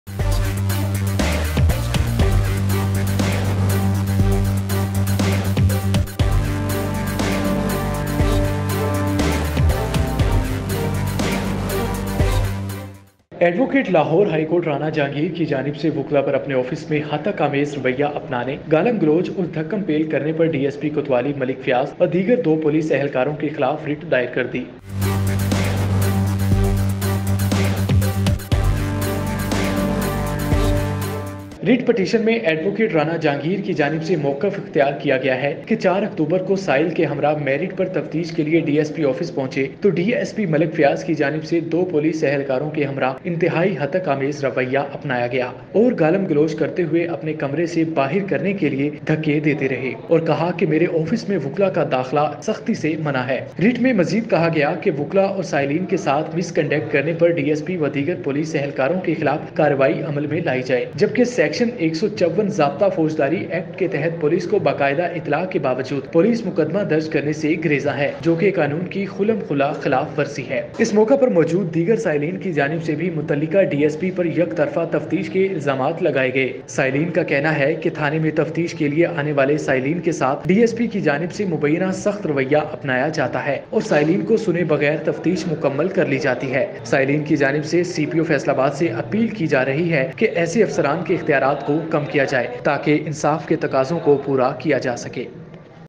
Writ was filed against DSP Kotwali and two other police officers on behalf of Advocate Rana Jahangir
Writ was filed against DSP Kotwali Malik Fayyaz and two other police officers on behalf of Advocate Rana Jahangir